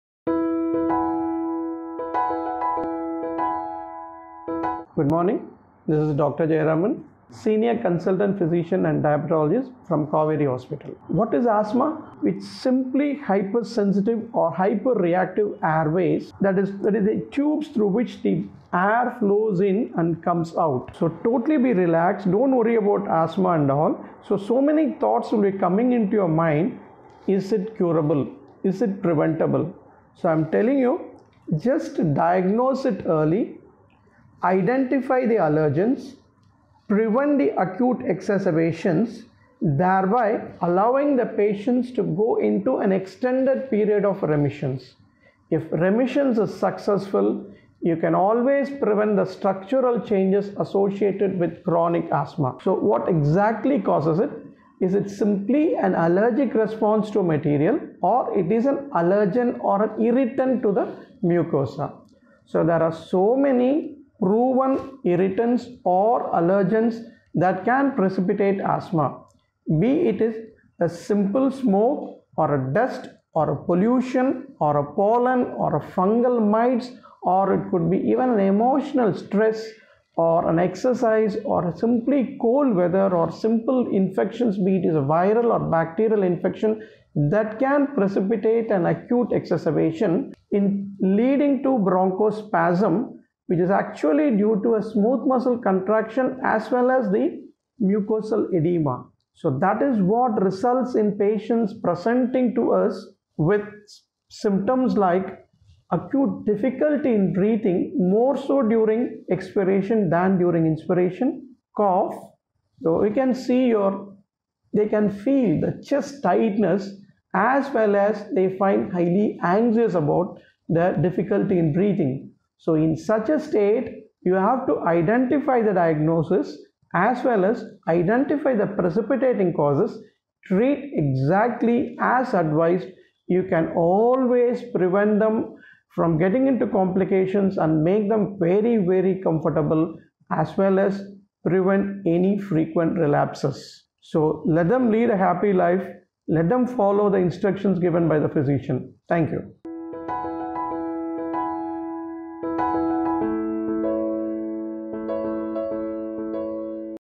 All about Asthma | Dr K Jayaraman | Kauvery Hospital Chennai
Dr K Jayaraman, Senior Consultant, Internal Medicine and Diabetology, Kauvery Hospital Chennai shares his insights about Asthma and how it can be managed.